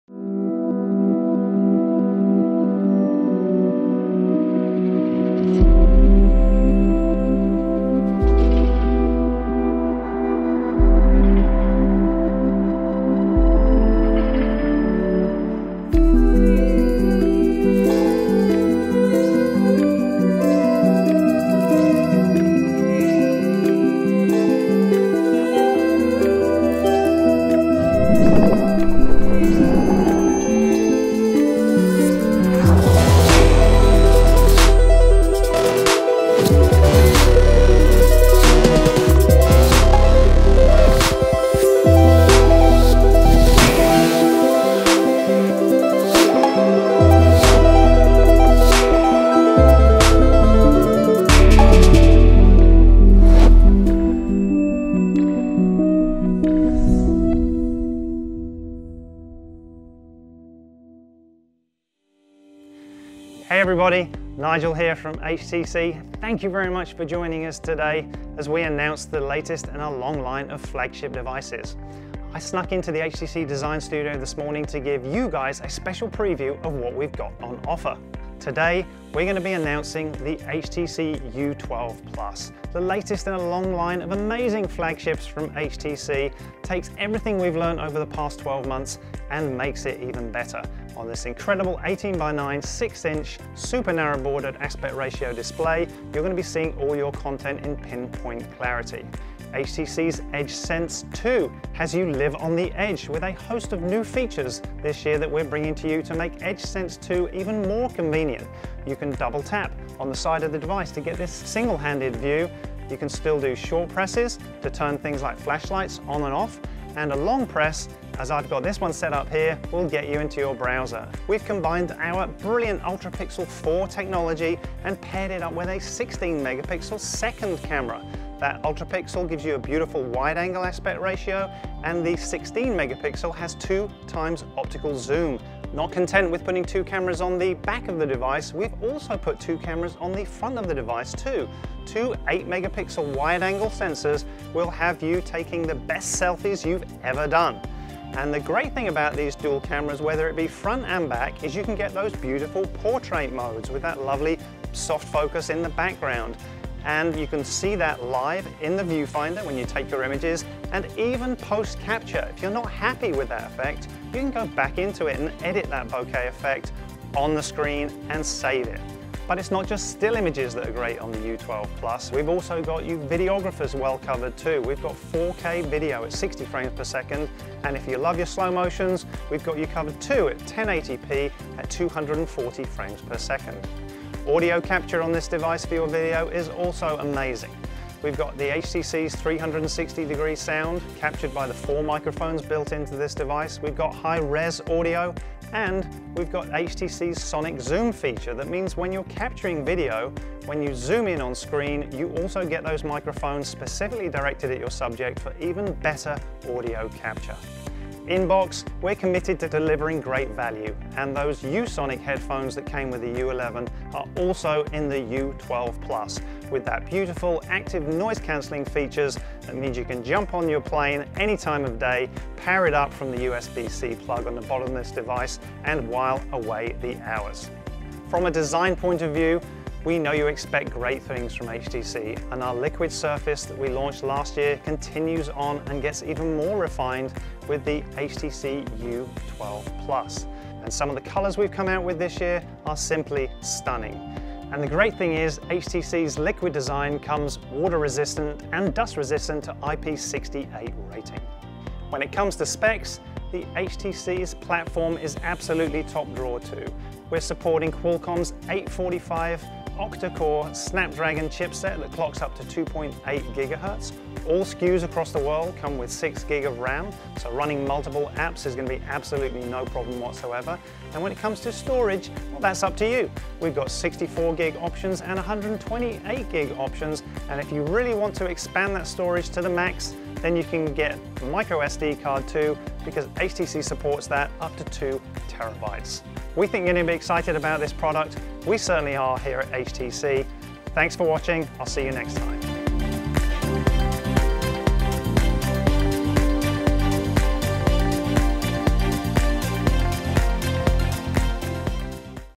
Bigger, Bolder and Edgier. Introducing the HTC U12+.
Meet the HTC U12+, our newest flagship device that introduces all new ways of interacting with your favorite features with Edge Sense 2, an improved camera experience and much more. Hosted by HTC VP of Product Nigel Newbyhouse.

Live on the Edge with the HTC U12+.

Learn more about the new HTC U12+ at htc.com